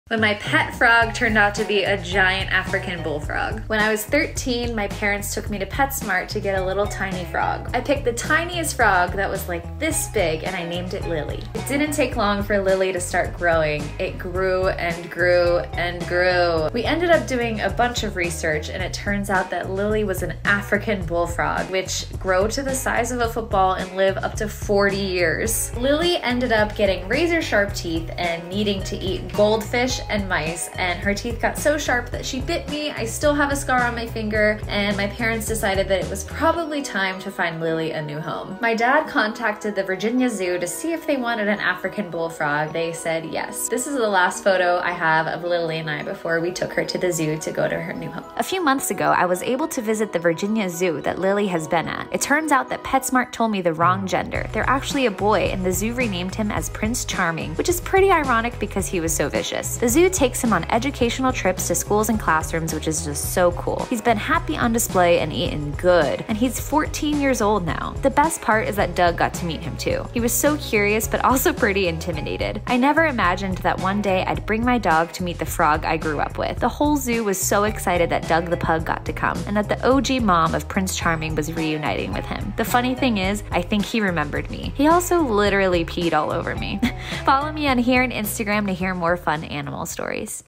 When my childhood pet frog turned out to be a GIANT African Bullfrog!
**I do not support buying animals from big pet stores! ALWAYS do your research first. This was almost 15 years ago...times were different, and my parents trusted the information that the employee gave us at the time. DO YOUR RESEARCH, too many pets end up in unsafe situations because of people blindly buying them and not being able to care for them!**